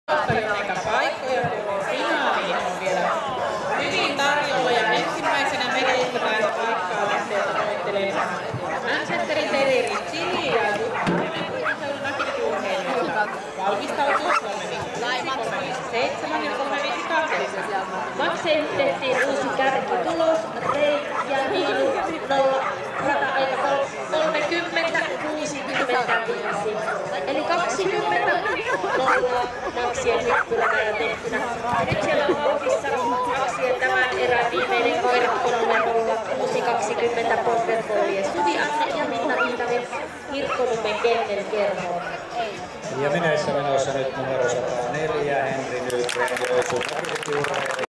M, karsinta , Chili
SM karsinta ( hyppäri ) , Medi , Manchesterinterrieri , Chili , rv 0 , Oulu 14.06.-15 , tuomari Jan Egil Eide ( NOR)